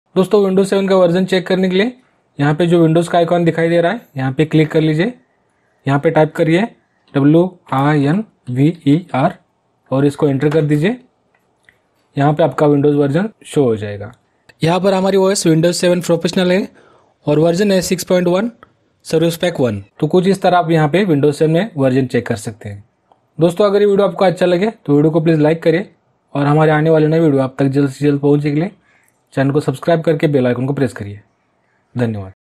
How To Check Windows 7 Version In Laptop | How To Check Windows Version In Hindi
नमस्कार दोस्तो, मेरा नाम है 'नितीन' और Technical Nitink Channel मे आपका स्वागत है | दोस्तो आज के इस व्हिडीओ मे मैने ' Windows 7 Version ' कैसे चेक करते है इसके बारे मे जानकारी दि है | दोस्तो आपका Laptop हो या Desktop इस व्हिडीओ के जरीये आप Windows 7 का Version चेक कर सकते है | दोस्तो  Windows 7 के साथ-साथ आप इस प्रोसेस से Windows 8 version, Windows 10 version , Windows 11 का version भी चेक कर सकते है | इसके अलावा आप CMD मे भी 'winver' type करके Windows version चेक कर सकते है | दोस्तो  उम्मीद है यह व्हिडीओ आपको पसंद आयेगी |

आपके काम मे आनेवाले व्हिडीओ |